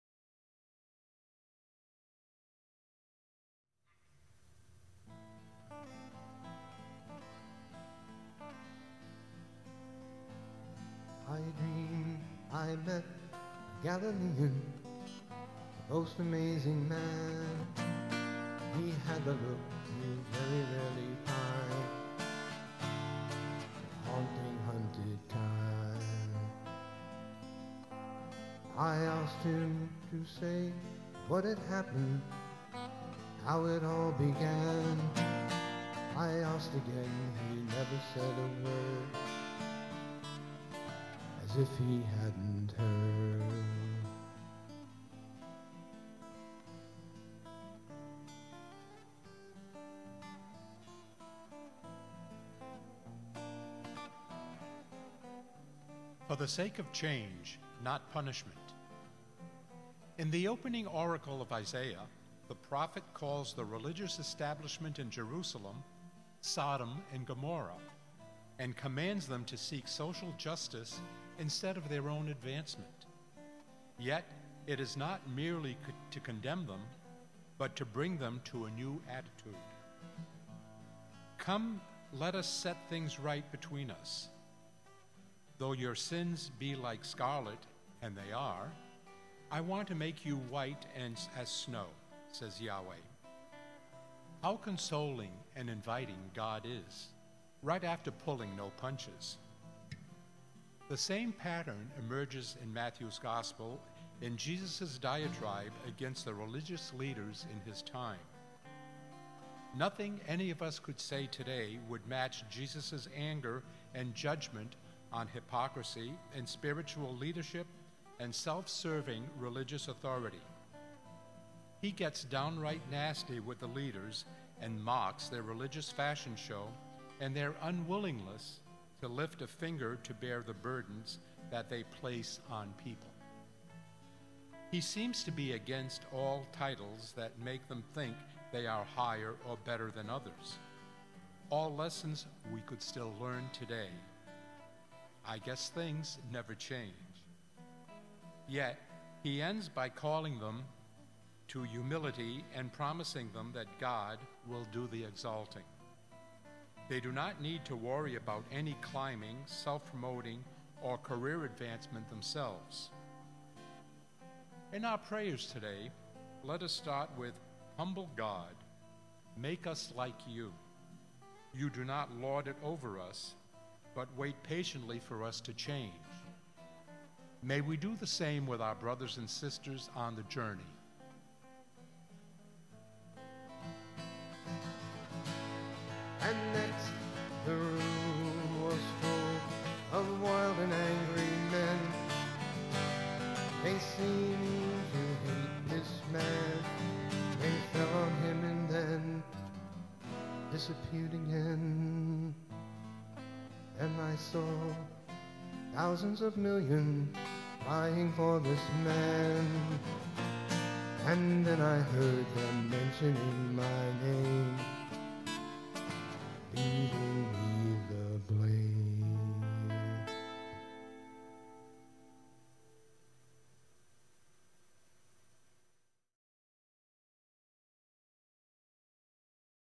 Digital Lent | Wednesday, March 16, 2022
Enjoy some Lenten music and a daily reflection with us during our Lenten Journey.  Readings are excerpts from Richard Rohr's book Wondrous Encounters - Scripture For Lent.
By the time you've finished your cup of coffee you'll have a new thought to ponder as we spend this time in fasting and prayer.
New reflections are available Mon - Thurs of Lent at 6:30am here on YouTube. On Fridays, please join us in person or online for Stations of the Cross.